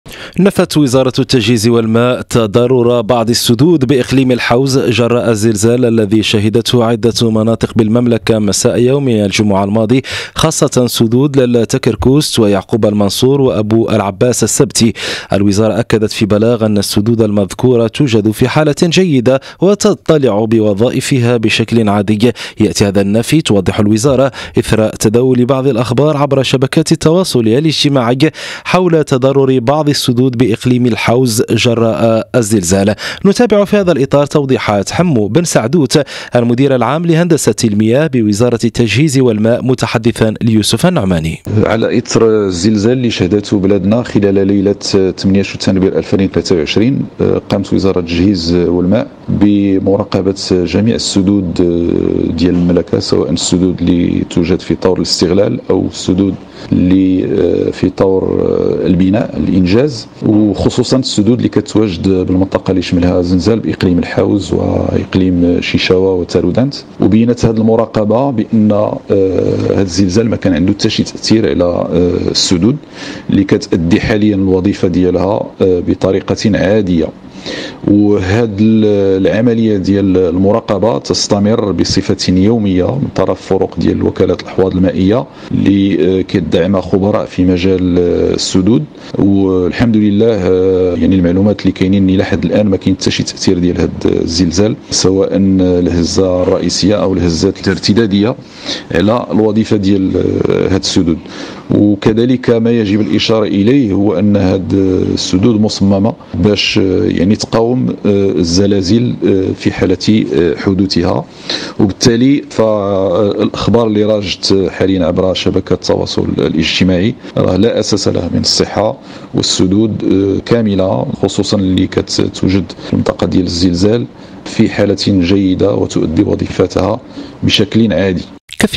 تضرر السدود..خبر زائف
حالة السدود بعد زلزال_الحوز يقدمها حمو بنسعدوت، المدير العام لهندسة المياه من الرباط.